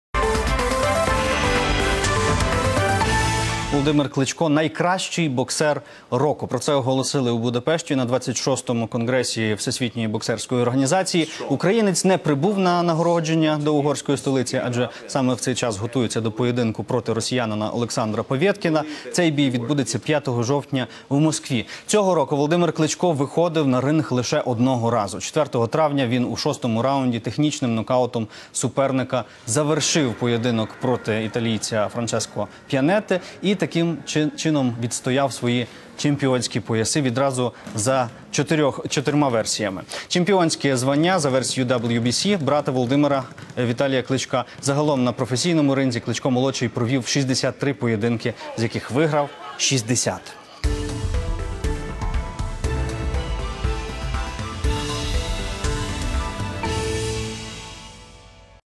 В Будапеште Кличко признали лучшим боксером года
Владимир Кличко - лучший боксер года. Об этом объявили в Будапеште на 26му конгрессе Всемирной боксерской организации

Володимир Кличко -- найкращий боксер року. Про це оголосили у Будапешті на 26му конгресі Всесвітньої боксерської організації